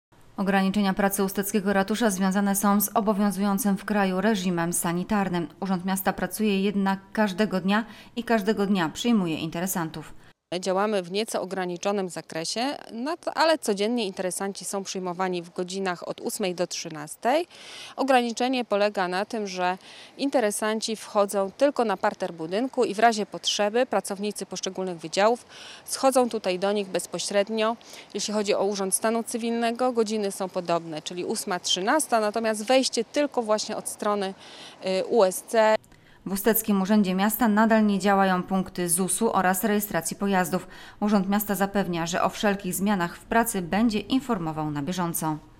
Ograniczenia w pracy ratusza - WIADOMOŚCI USTKA | Obserwator 28.08.2020
Ustecki magistrat nadal pracuje z pewnymi ograniczeniami. Jednak interesanci przyjmowani są każdego dnia przy zachowaniu zasad ostrożności.

Więcej wiadomości z Ustki na naszym kanale TV Słupsk. Zachęcamy do subskrypcji i klikania dzwoneczka, by być z nami na bieżąco.

Obserwator dla TV Słupsk w sieci Vectra 2020